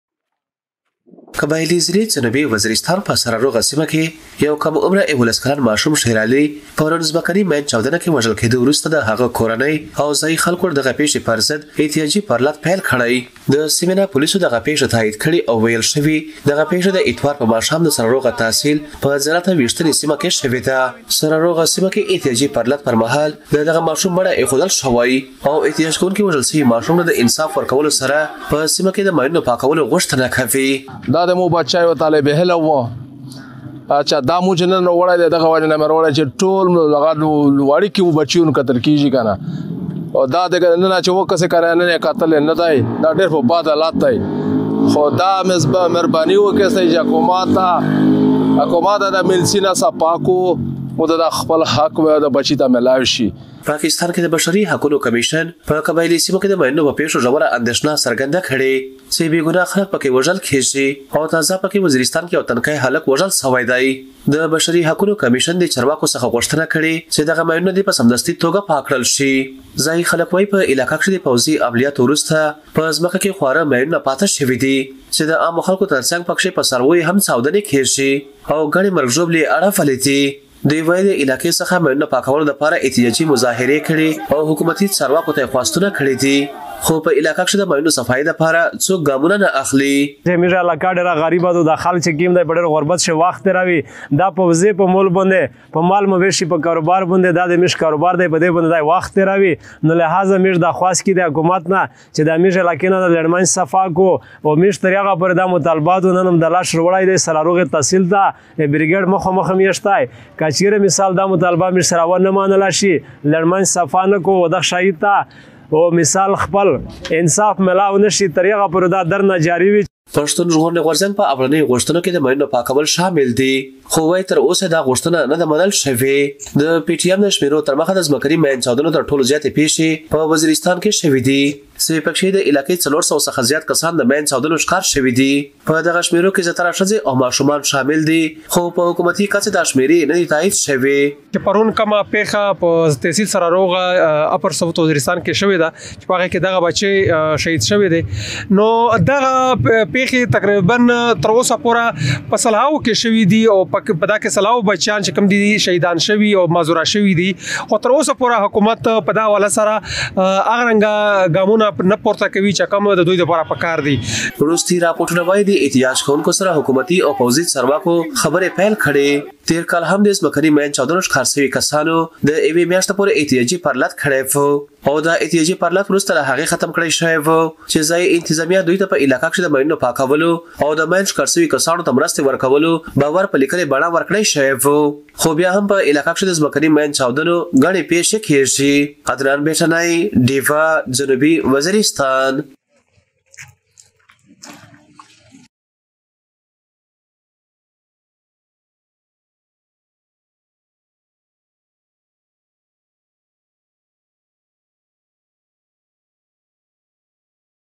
جنوبي وزیرستان: په ماين چاودنه کې یو ماشوم وژل شوی، ځايي خلکو احتجاج پیل کړی
د افغانستان سره په پوله پرتې د خېبرپښتونخوا په قبايلي ضلع بره جنوبي وزيرستان کې په یوه ماين چاودنه کې يوولس کلن ماشوم وژل شوی دی. ځايي خلکو یو وارې بیا احتجاجونه پېل کړي. نور تفصیل د عدنان بیټني څخه په دې راپور کې: